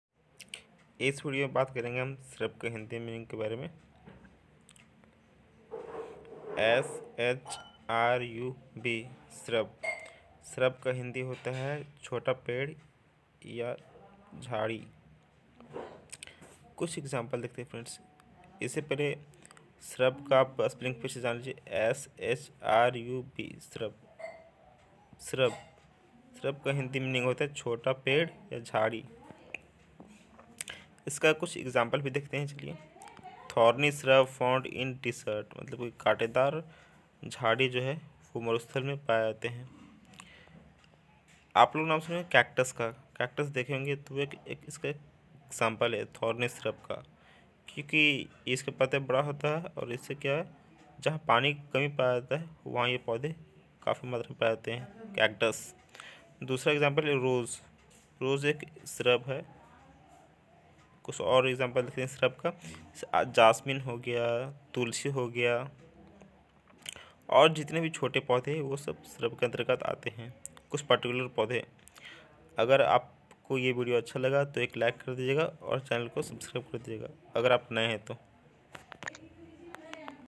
Shrub meaning in hindi
Is video mein ham baat karenge shrub ka meaning aur kuchh example k saath.

shrubs ka hindi